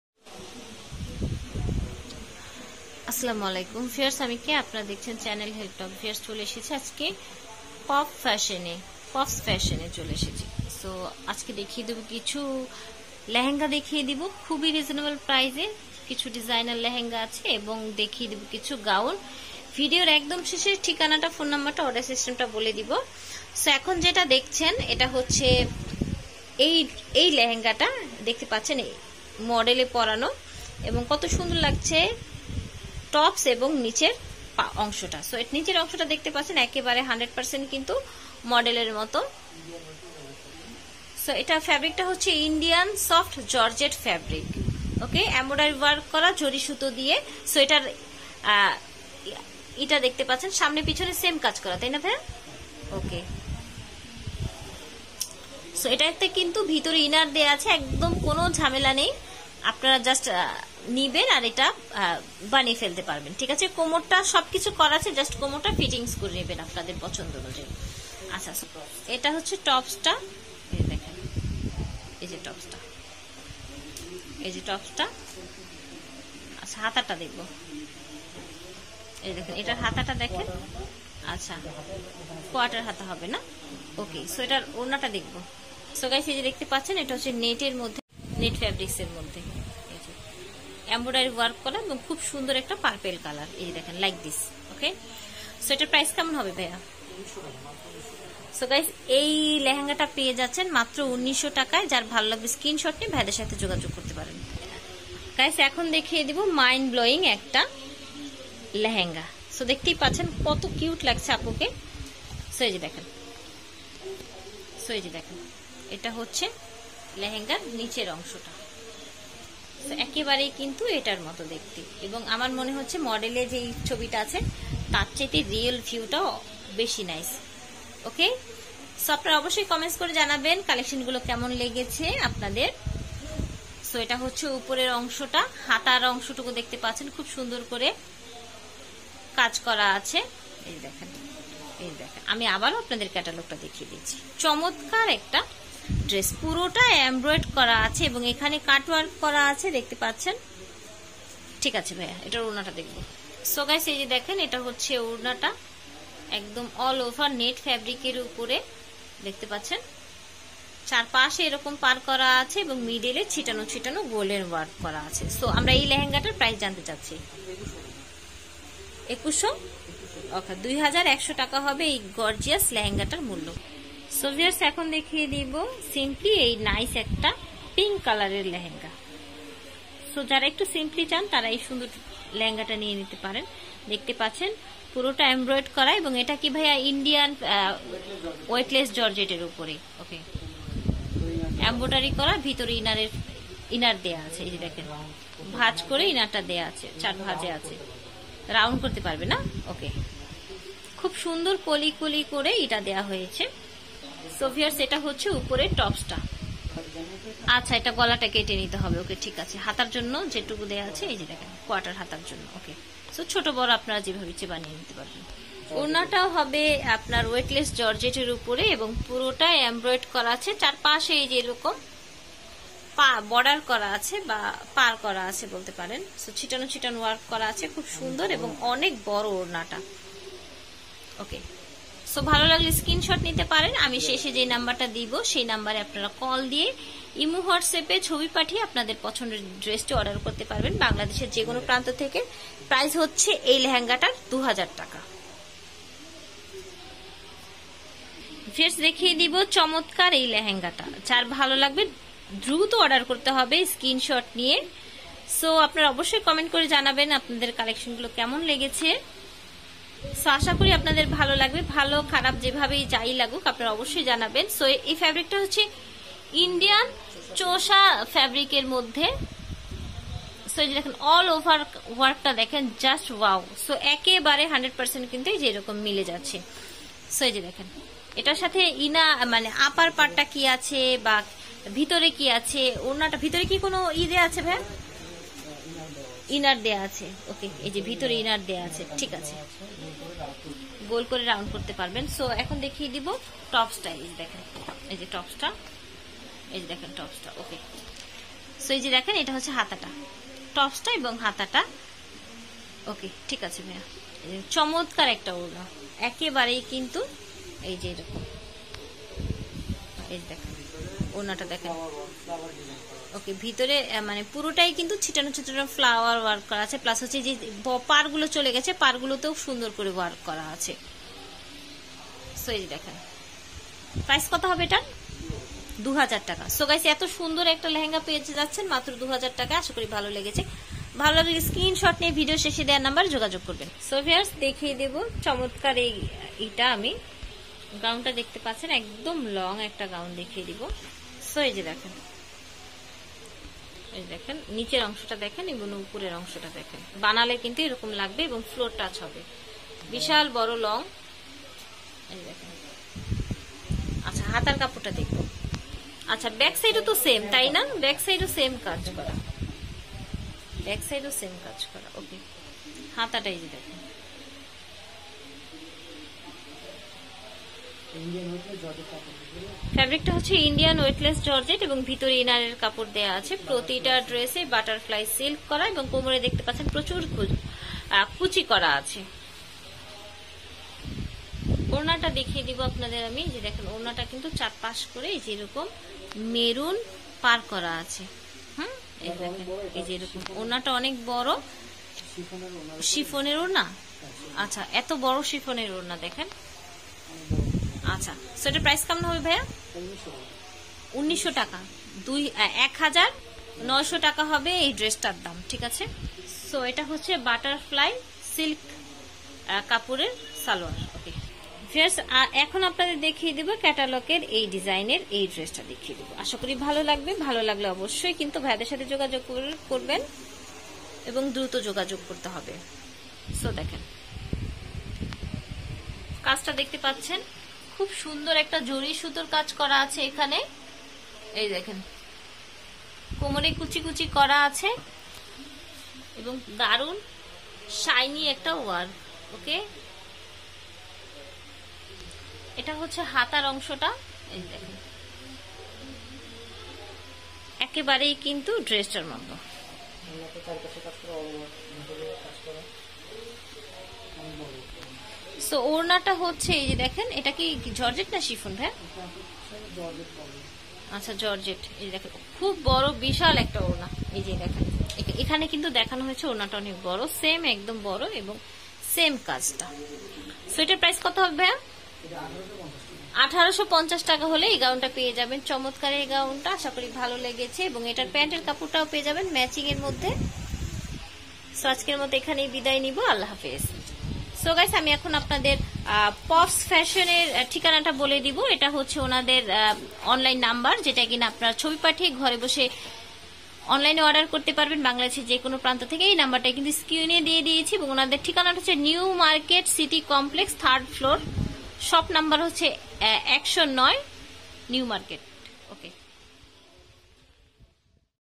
অসম্ভব কমদামে ডিজাইনার লেহেঙ্গা/স্টুডেন্ট বাজেটে ঈদের জন্য মাথা নষ্ট লেহেঙ্গা কালেকশন | HELP TALK
2021_EID_COLLECTION_BD